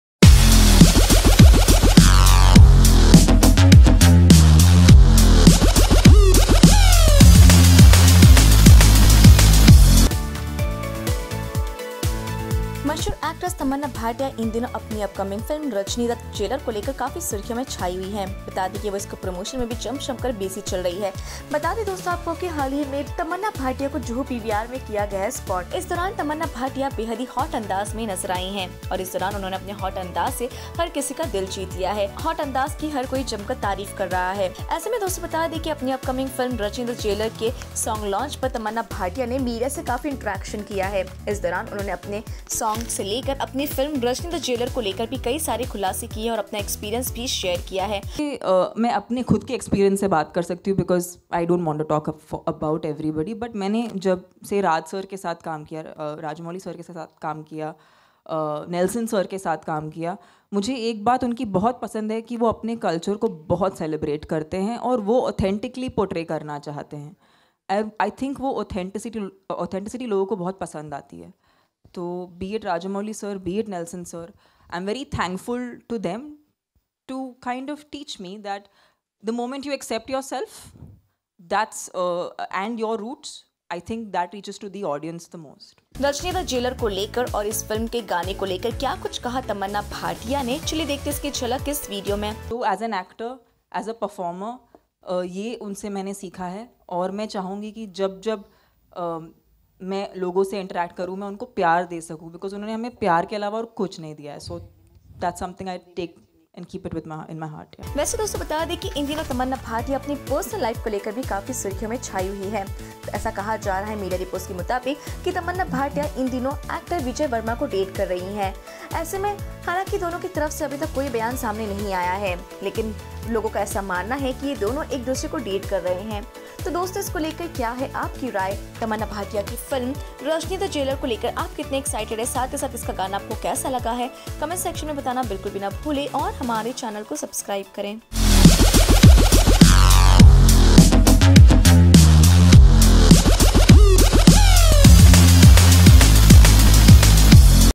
Tamannaah Bhatia sizzles in a hot avatar at a song launch | Bollywood News | ErosNow eBuzz
Setting the temperature soaring high, Tamannaah Bhatia sizzles yet again at a launch event in Mumbai.